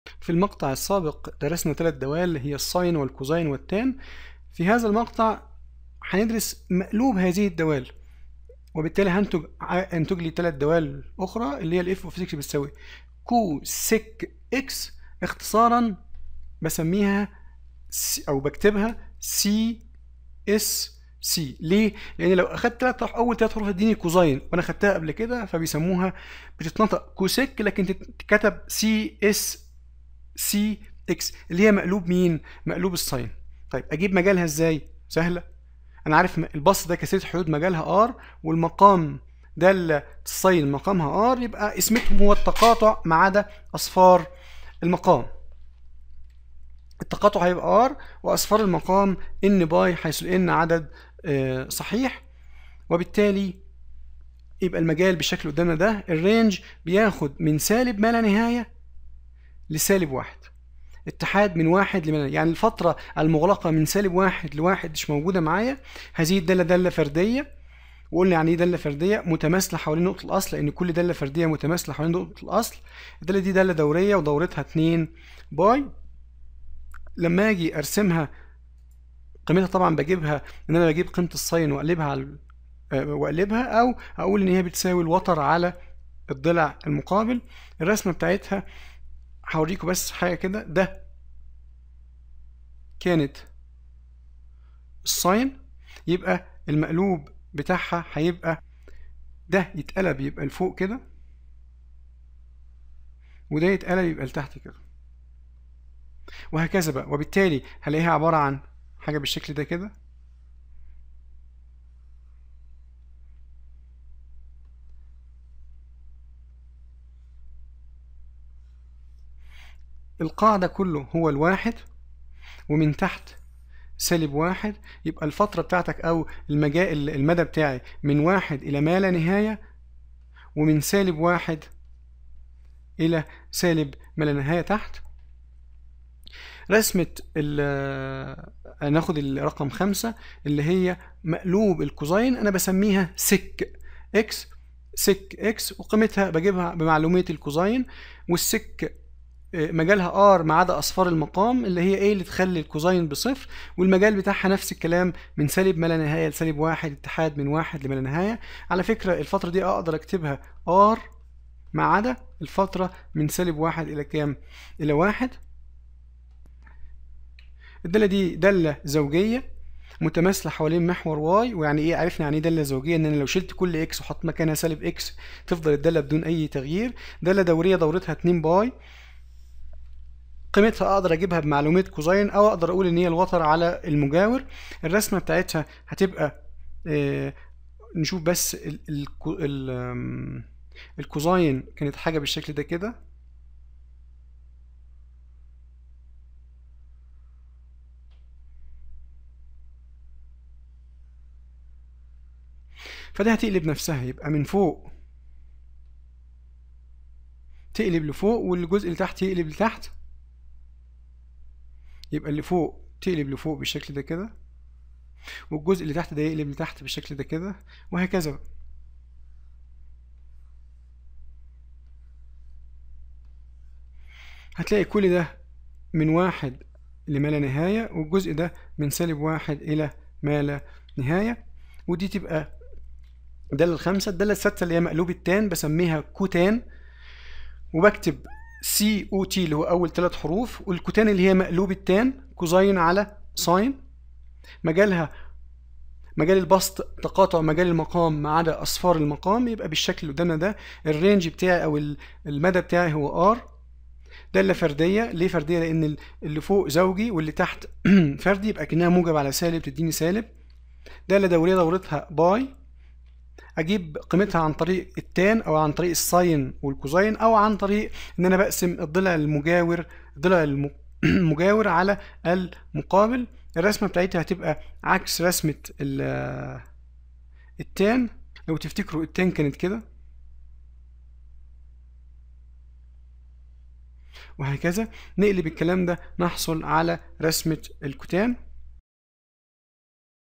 حساب التفاضل والتكامل - الوحدة 2 : الدالة المثلثية - 2 - Tri
المقررات المفتوحة
حساب التفاضل والتكامل(1) - 101 ريض
الوحدة الثانية - الدوال الحقيقية
باقي الدوال المثلثية، مجالها، مداها، خواصها، شكلها.